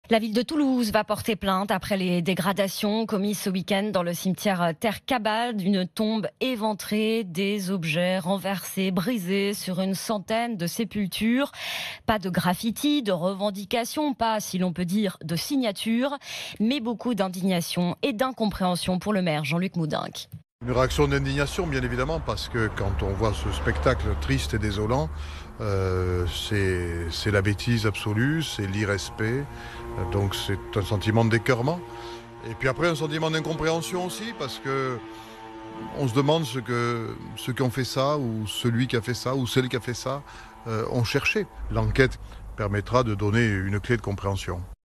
Cimetière vandalisé à Toulouse: "Un spectacle triste et désolant"
A Toulouse, une centaine de tombes ont été dégradées dans la nuit de samedi à dimanche. Une enquête est en cours.